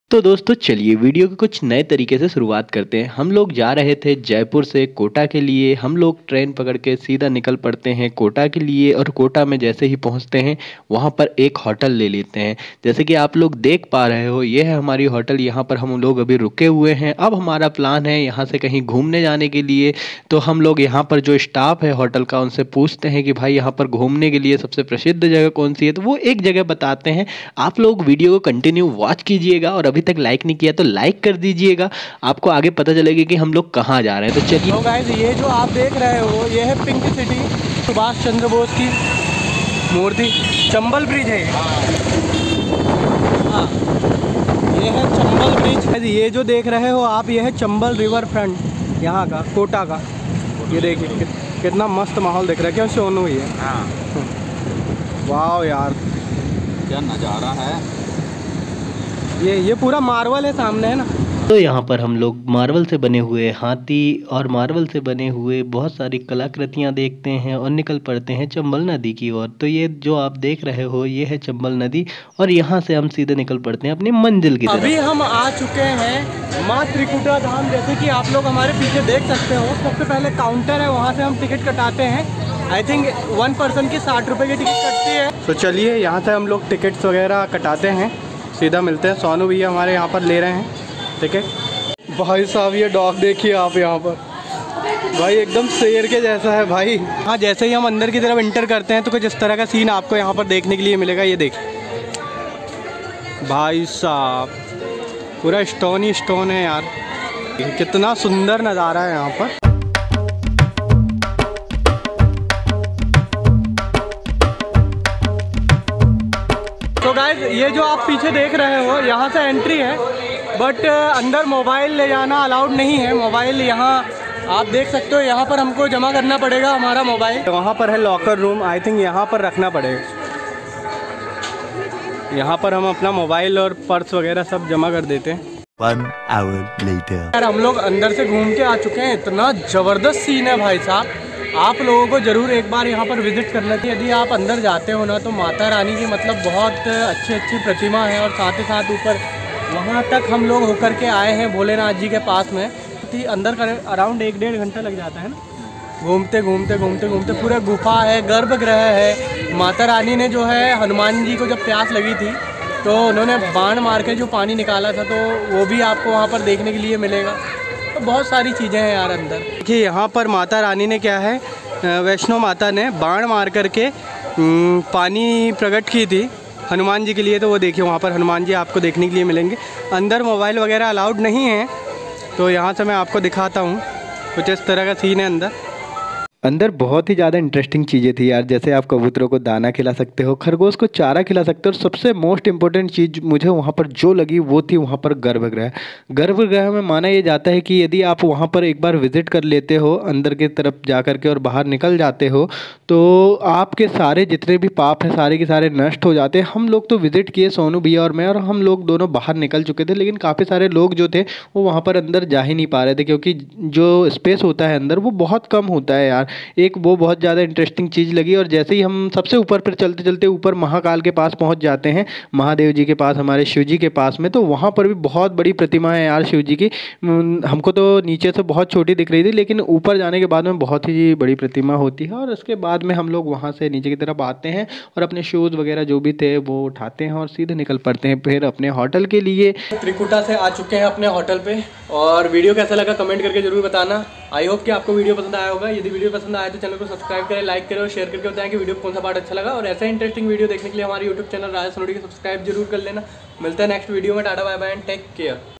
कोटा में यह जगह नहीं देखी तो क्या देखा,पहाड़, गुफाएं, झरने / Maa Vaishno Devi Yatra Trikuta Kota
About This Video:- राजस्थान के कोटा में स्थित मनमोहक स्थल में से एक मां त्रिकुटा धाम जो मां वैष्णो देवी का मंदिर है जहां पर आपको पहाड़ गुफाएं और झरने इस वीडियो में देखने के लिए मिलेंगे साथ ही आपको कोटा की पिंक सिटी और मार्बल से बनी हुई विभिन्न कलाकृतियां साथ ही चंबल रिवर फ्रंट और चंबल नदी इस वीडियो में देखने के लिए मिलेगी |